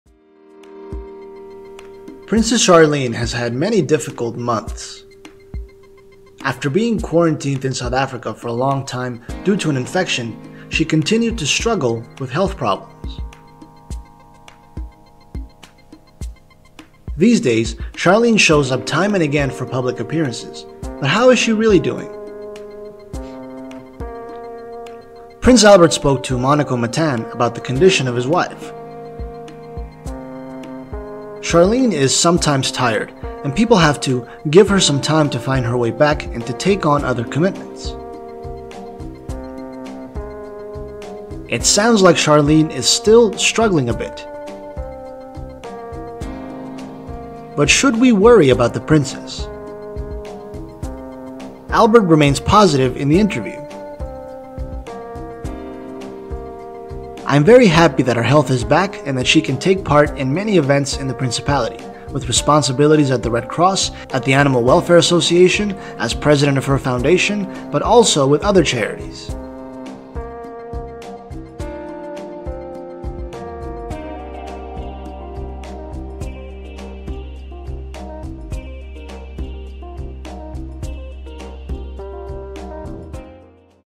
Sad Truth: Prince Albert Gives Update On How Charlene Is Really Doing | ROYAL FLAIR
Princess Charlène has had a difficult time as of late. She had to withdraw for a long time because she was struggling with health problems. Now she's making public appearances again, but is she really alright?.